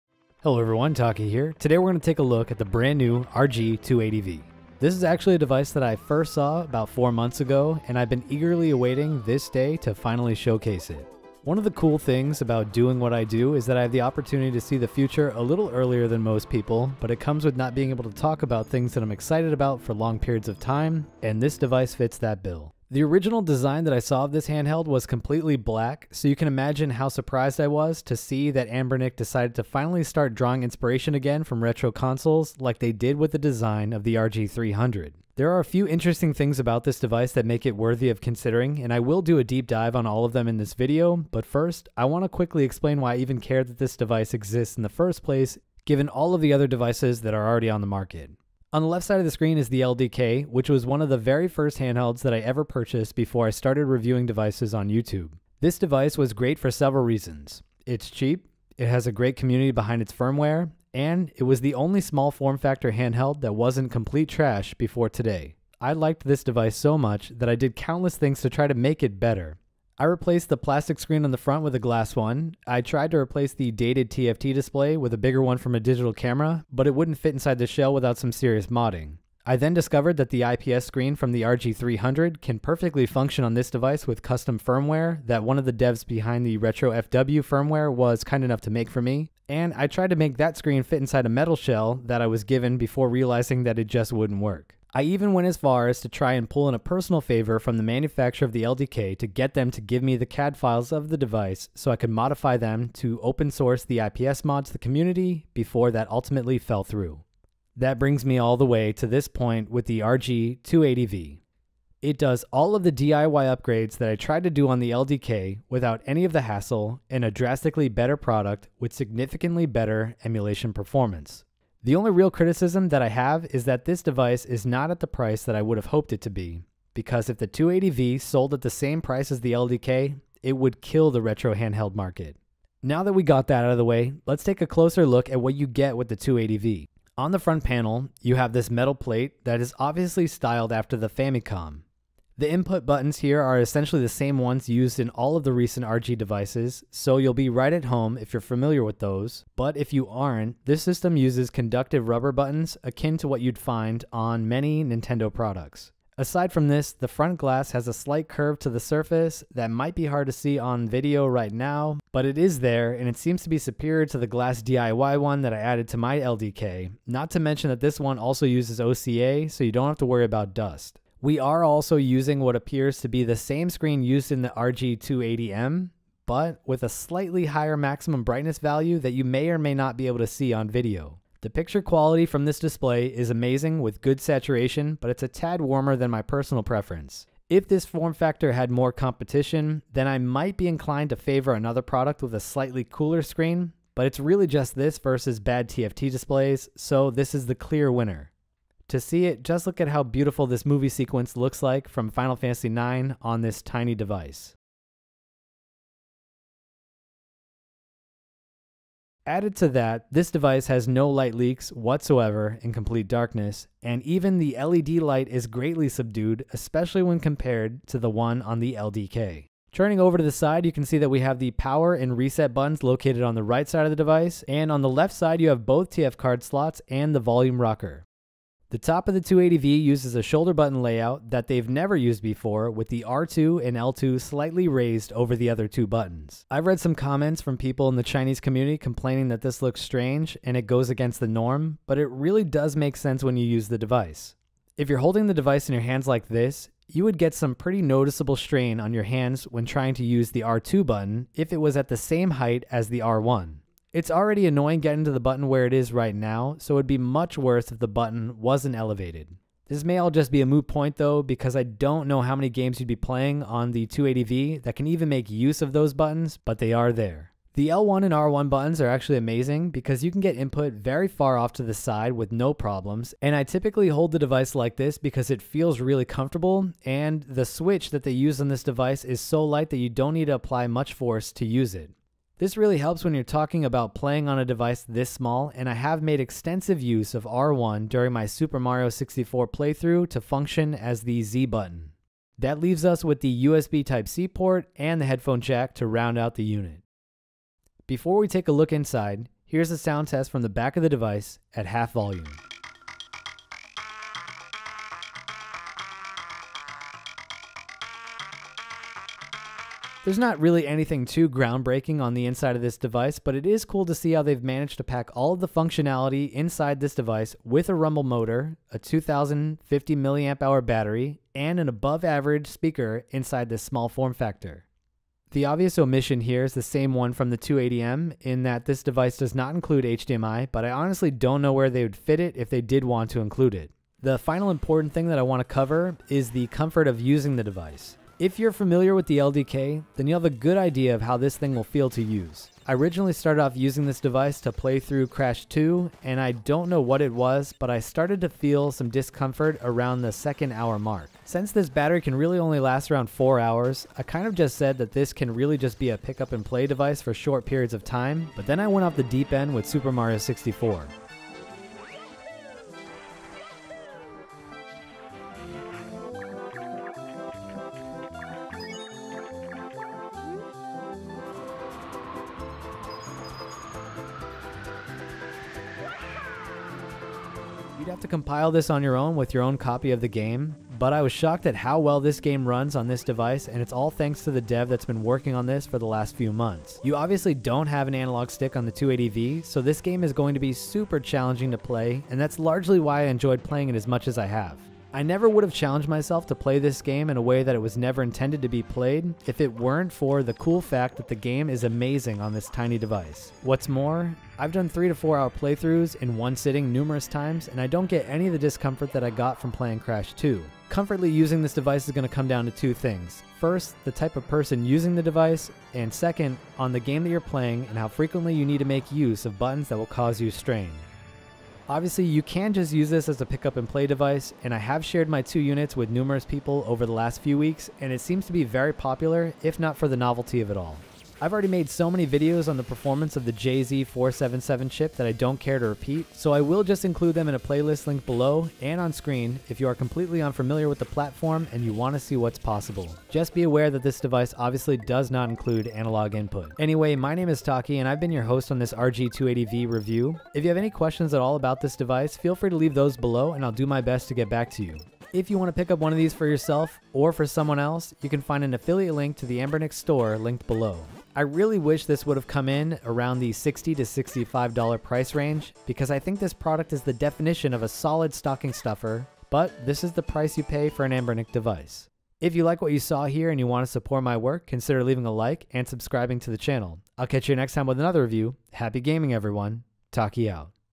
This Device is Amazing! RG280V Famicom Edition
There's a new contender for the best small form factor emulation handheld and it comes with Famicom colors! The RG280V is the latest device from Anbernic running on their JZ4770 platform. In this video, we take a look at the RG280V to see if it is worth your time and money.